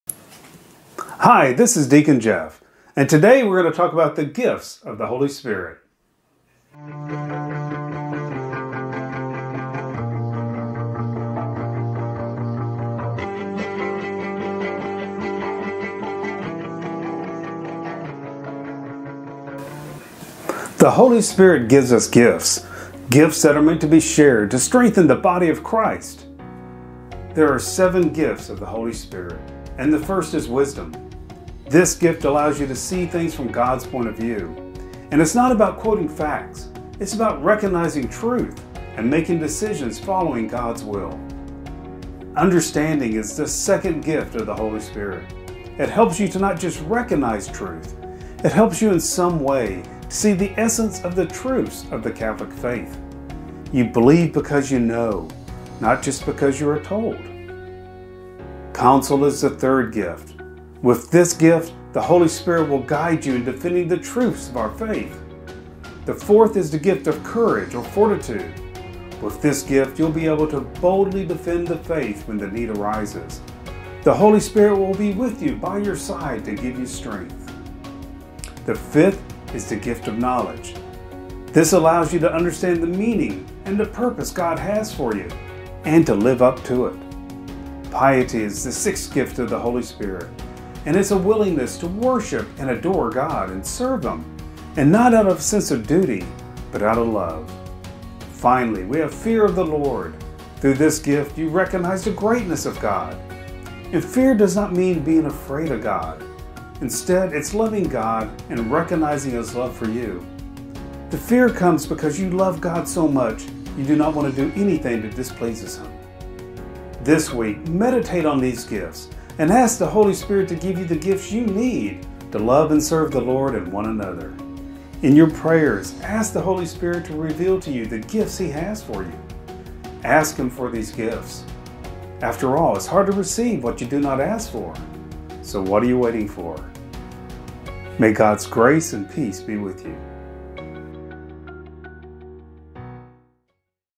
Gifts of the Holy Spirit
The Holy Spirit gives us gifts. Gifts that are meant to be shared to strengthen the Body of Christ. There are seven gifts of the Holy Spirit. These gifts are:

- Wisdom
- Understanding
- Counsel
- Courage
- Knowledge
- Piety
- Fear of the Lord.

In this video, we will briefly talk about each of these gifts. Meditate on these gifts and ask the Holy Spirit to give you the gifts you need to love and serve the Lord and one another. In your prayers, ask the Holy Spirit to reveal to you the gifts He has for you. Ask Him for these gifts. After all, it is hard to receive what you do not ask for. So what are you waiting for?